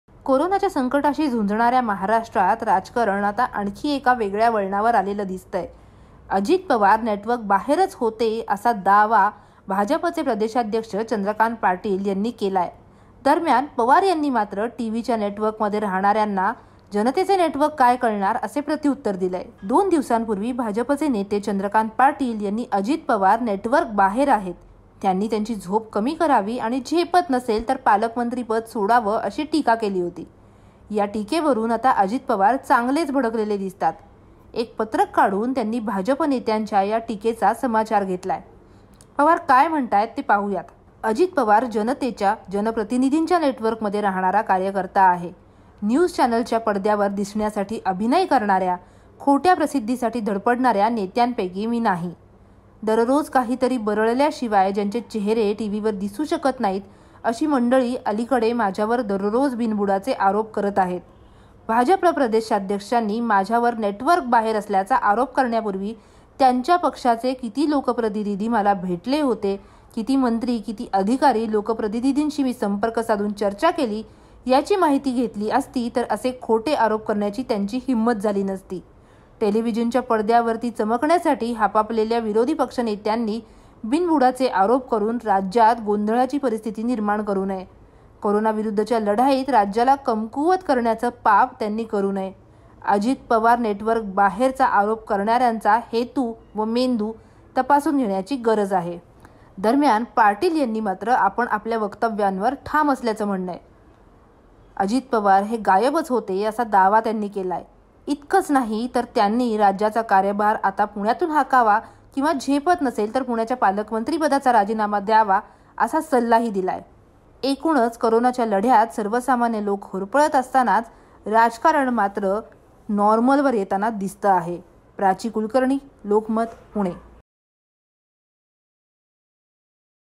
कोरोनासोबतच राजकीय आरोप प्रत्यारोप | Ajit Pawar VS Chandrakant Patil | Maharashtra News
दोन दिवसांपूर्वी भाजपचे नेते चंद्रकांत पाटिल यांनी अजित पवार नेटवर्क बाहेर आहेत. त्यांनी झोप कमी करावी. झेपत नसेल तर पालकमंत्रीपद सोडावं अशी टिका केली होती. या टिकेवरुन आता अजित पवार चांगलेच भडकलेले दिसतायत. एक पत्रक काढुन त्यांनी भाजपनेत्यांच्या या टिकेचा समाचार घेतला. अजित पवार जनतेच्या नेटवर्कमध्ये राहणारा कार्यकर्ता आहे. न्यूज चॅनलच्या पडद्यावर दिसण्यासाठी अभिनय करणाऱ्या, खोट्या प्रसिद्धीसाठी धडपडणाऱ्या नेत्यांपैकी मी नाही. दररोज काहीतरी बरळल्याशिवाय ज्यांचे चेहरे टीव्हीवर दिसू शकत नाहीत, अशी मंडळी अलिकडे माझ्यावर दररोज बिनबुडाचे आरोप करत आहेत